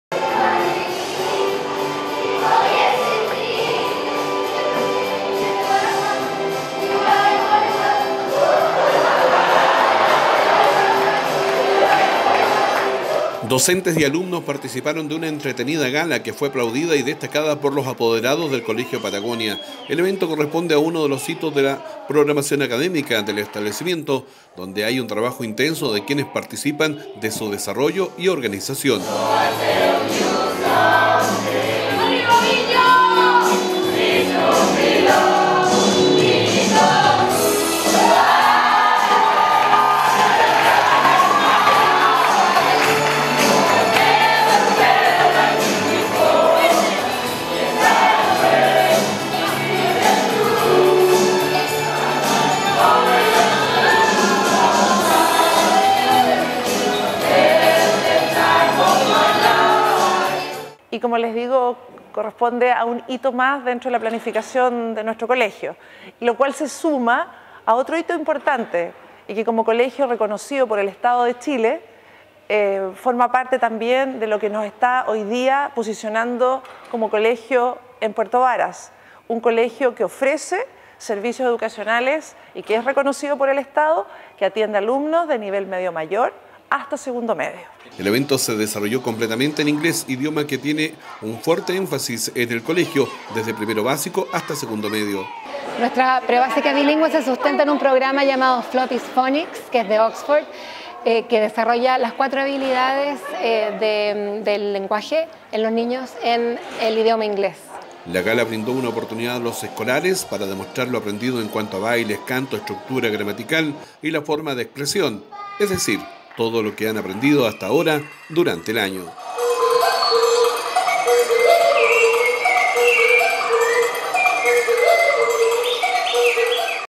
Colegio Patagonia efectúa gala en su totalidad en ingles.
Docentes y alumnos participaron de una entretenida gala que fue aplaudida y destacada por los apoderados del colegio Patagonia.